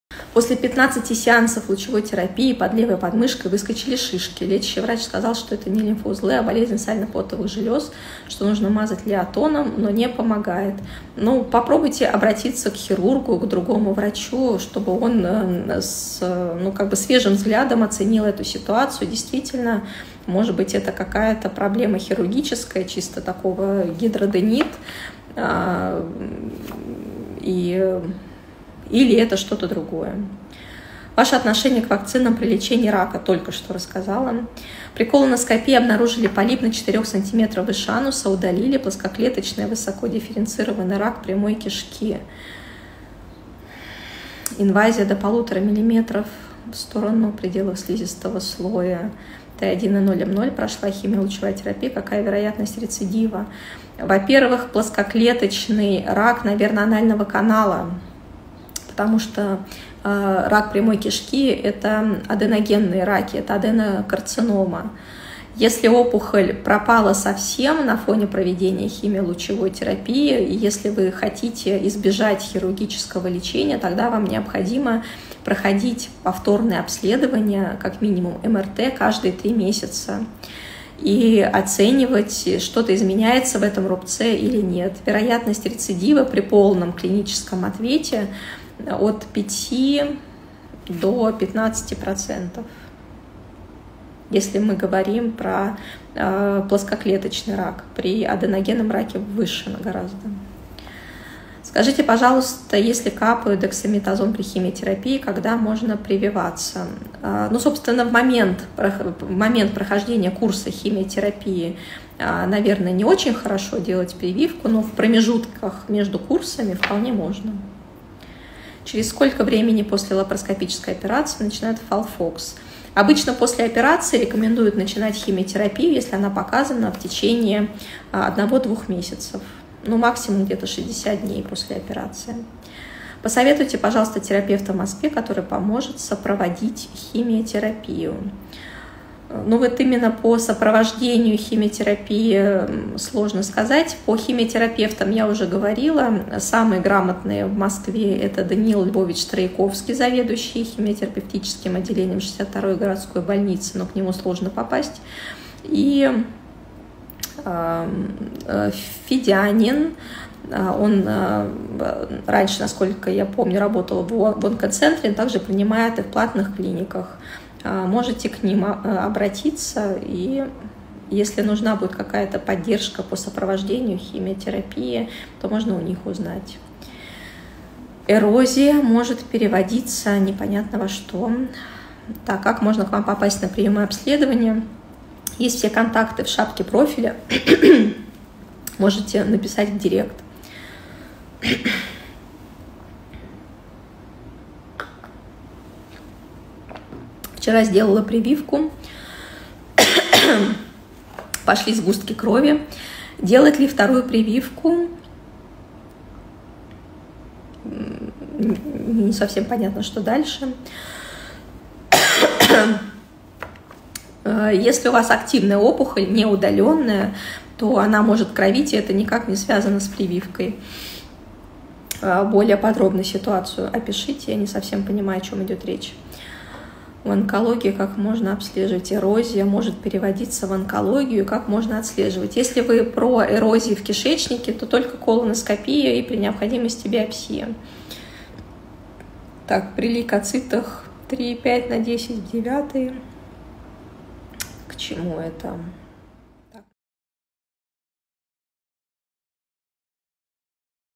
Химиотерапия: побочные эффекты, когда начинать. Может ли эрозия перейти в онкологию
Честные ответы на ваши вопросы по поводу рака кишечника.
Задать свой вопрос можно у меня в Инстаграме,